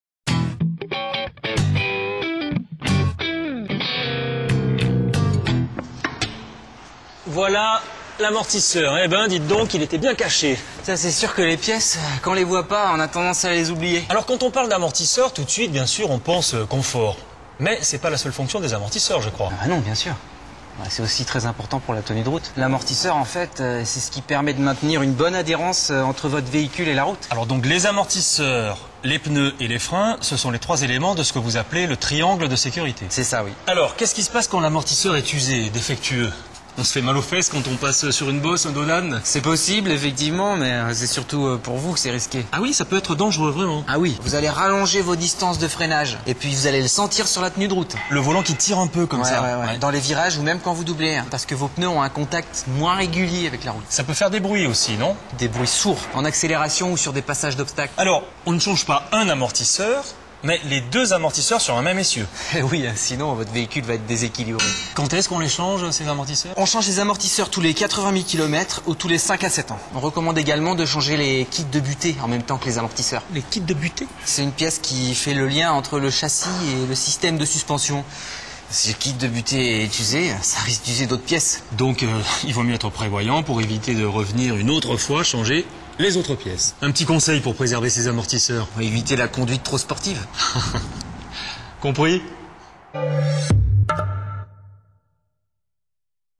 Renault Châteauroux - Minute Entretien #1 - Amortisseurs
Les amortisseurs sont peu visibles et sont trop souvent oubliés lors de l’entretien de votre véhicule. Leur usure graduelle fait que vous vous habituez aux performances amoindries de votre véhicule, de ce fait, vous n’êtes quasiment jamais alerté sur leur état de dégradation, voire du dépassement du seuil de sécurité. Il est donc important de connaître leur rôle et de savoir détecter les symptômes caractéristiques de leur usure :
   • Une distance de freinage rallongée
   • Une usure anormalement rapide de vos pneus
   • Une sensibilité accrue à l’aquaplaning par temps de pluie
   • Votre voiture cogne quand vous passez sur un « dos d’âne »

Si vous constatez l'un de ces signes, faites contrôler vos amortisseurs dans l'un de nos ateliers. Nos experts sont là pour vous conseiller.

Pour conserver le plus longtemps possible vos amortisseurs, nous vous recommandons :
   • De les faire contrôler à chaque révision
   • De les faire changer par paire tous les 80 000km
   • D’effectuer un contrôle de la géométrie lors d’un changement d’amortisseurs
   • De remplacer les kits butée de suspension en même temps que vos amortisseurs - plus de sécurité et pas de main d'oeuvre supplémentaire.